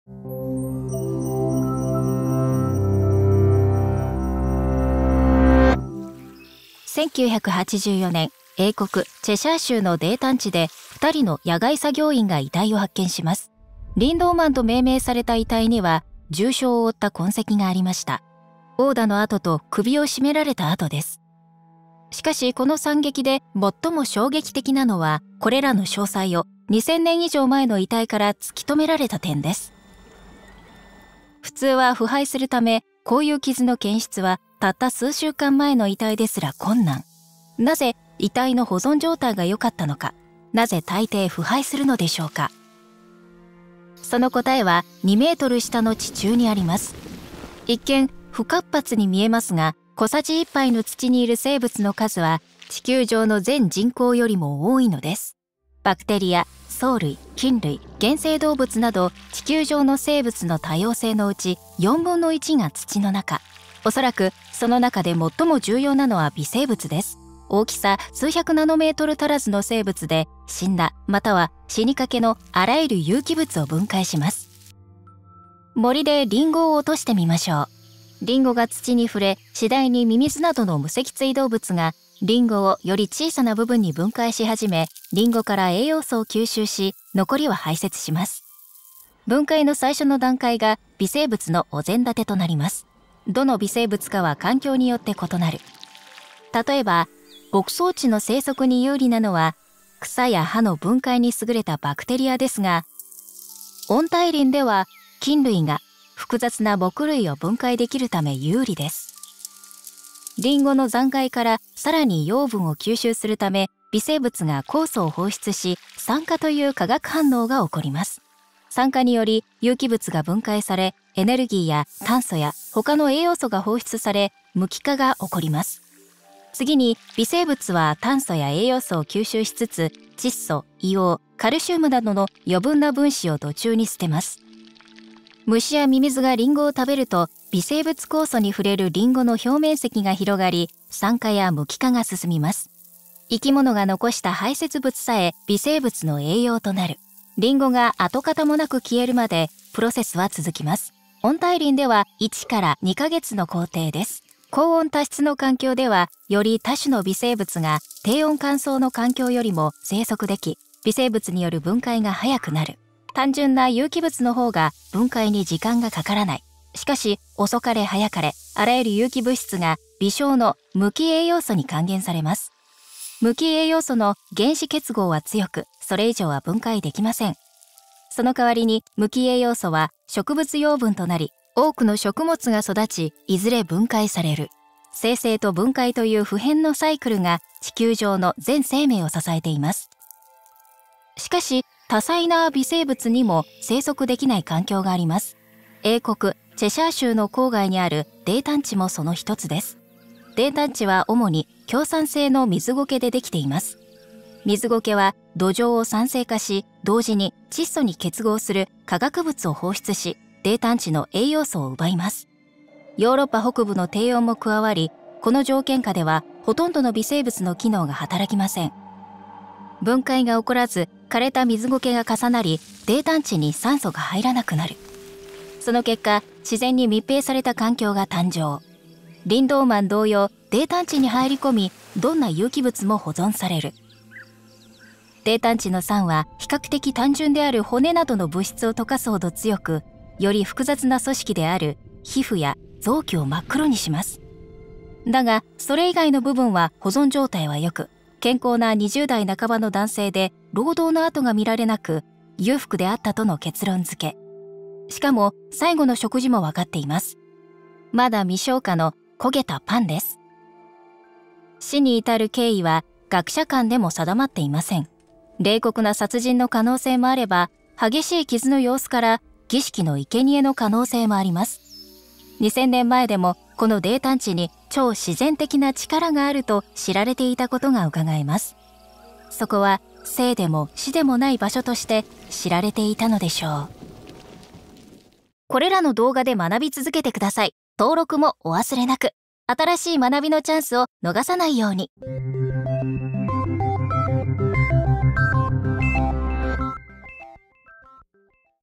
2000年前の死体が腐食しなかった理由とは？キャロリン・マーシャル
土がもつ驚くべき生物多様性と、そこにいる微生物が、いかに地球上のすべての生命を支えているかを知ってください。地下6フィートで、一見、不活発にみえますが、小さじ一杯の土にいる生物の数は、地球上の全人口よりも多いのです。バクテリア、藻類、菌類、原生動物など、地球上の生物の多様性のうち4分の1が土の中に棲息しています。そして、土に棲息する最も重要な生物は、おそらく微生物でしょう。キャロライン・マーシャルが、地球上のすべての生命をサポートするうえで、土がどれほど目に見えない助けになっているかを解明します。

Carolyn Marshall によるレッスン、Ivana Bošnjak, Thomas Johnson 監督。